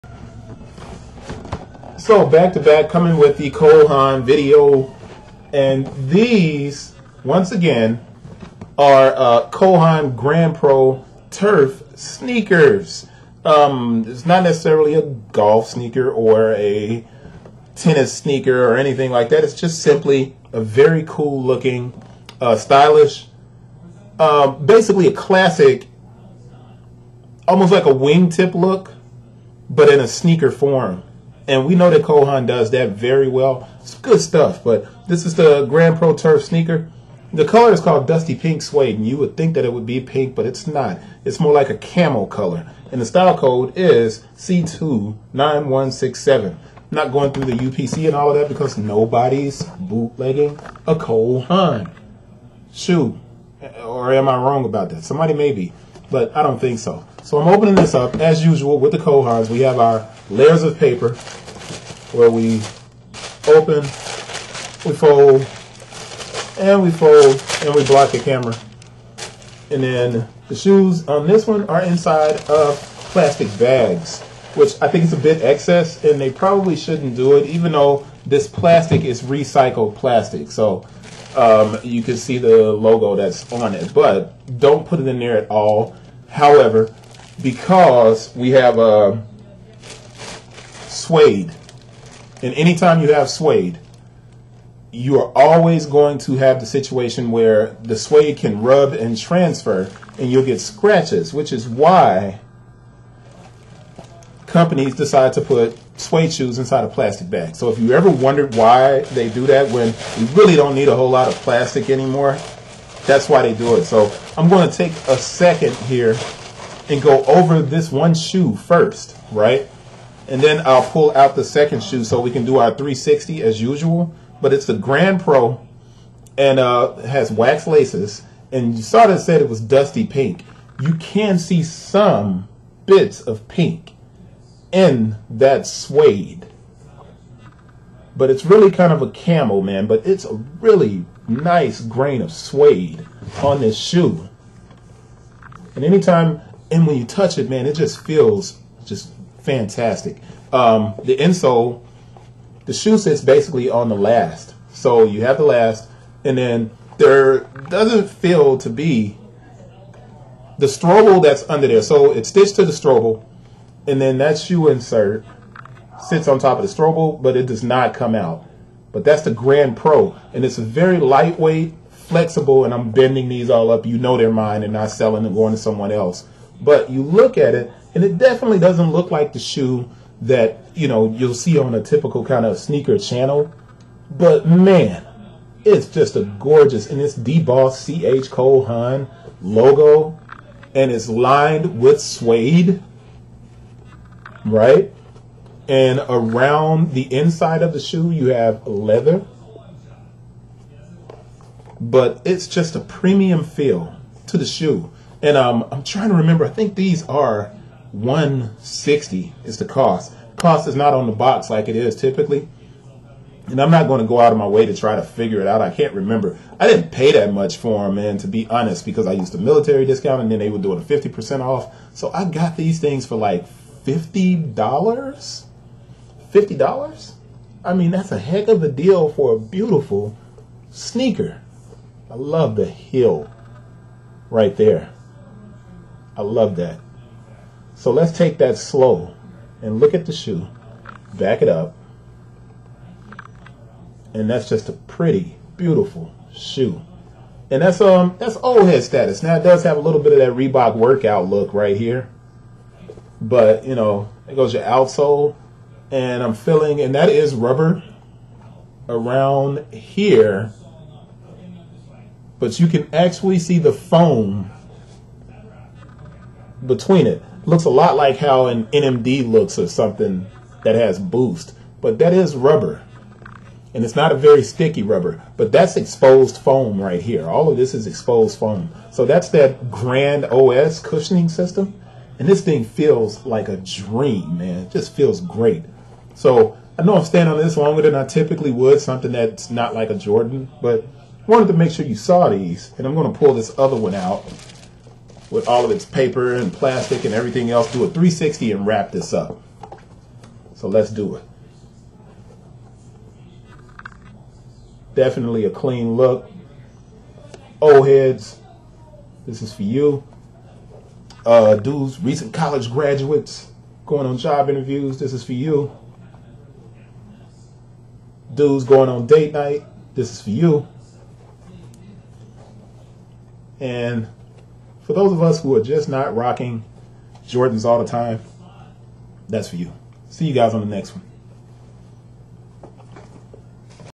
Cole Haan GrandPro Turf Sneaker 'Dusty Pink Suede' C29167
The Cole Haan GranPro features an almost adidas NMD styled outsole. The rubber outsole exposes the Grand OS midsole cushioning. The upper is a butter smooth suede and leather with a soft suede and leather lined interior. It's definitely a premium feel from Cole Haan.